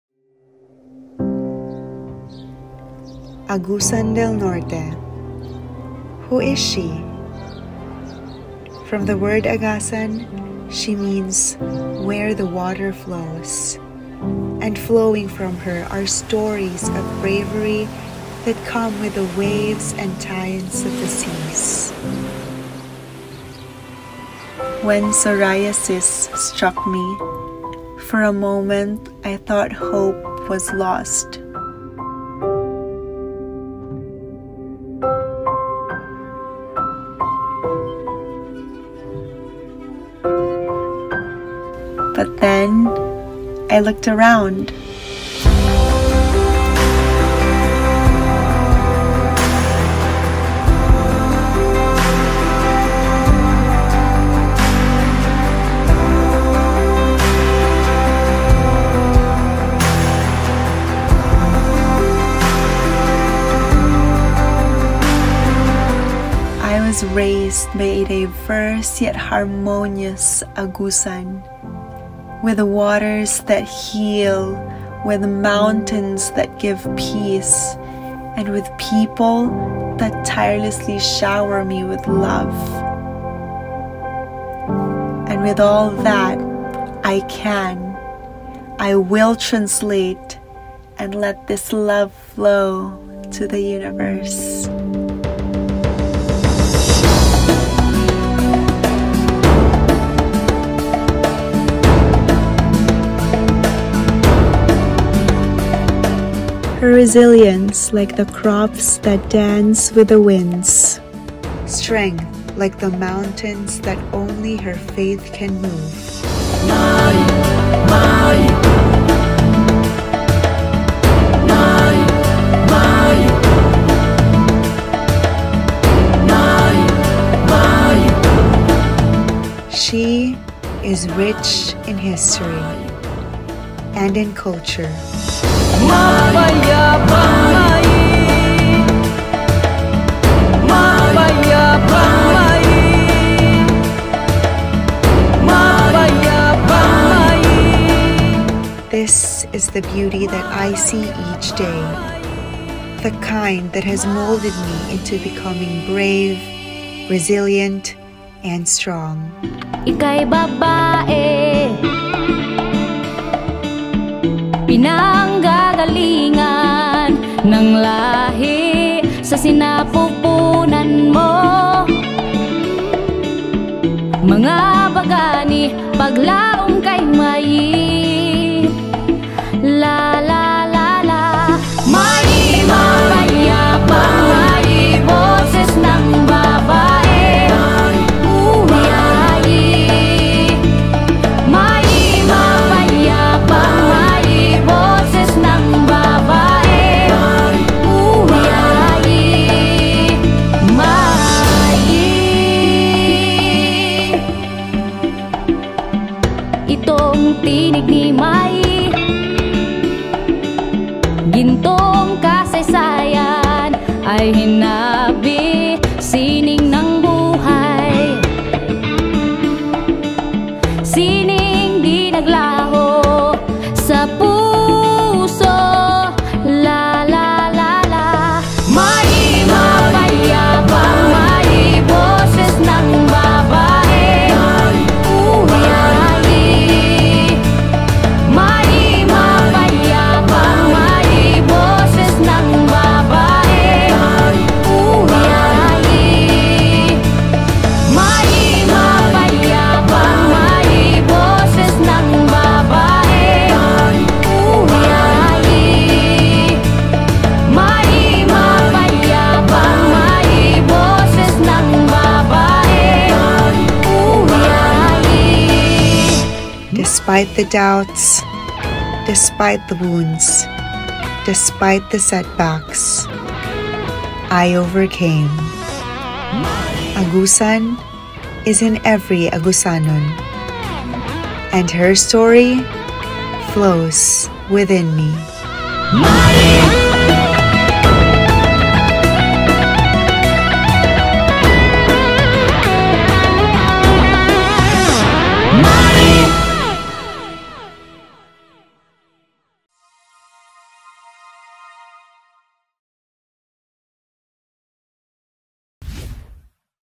HER STORY ft. Jannarie Zarzoso x Agusan Del Norte | Miss Universe Philippines 2023
“When psoriasis struck me, for a moment, I thought hope was lost…”

Watch as Jannarie Zarzoso tells HER STORY featuring the province of AGUSAN DEL NORTE and Cabadbaran City.

Screenplay Writer: Ann Mae Monique R. Jongko
Cinematography: Collide Films
Styling & Wardrobe: King Niño Aranchado
MUA: Junel Hinayon
Hair Stylist: Von Medelo
Special thanks to: Province of Agusan del Norte Tourism Office and Cabadbaran City Tourism Office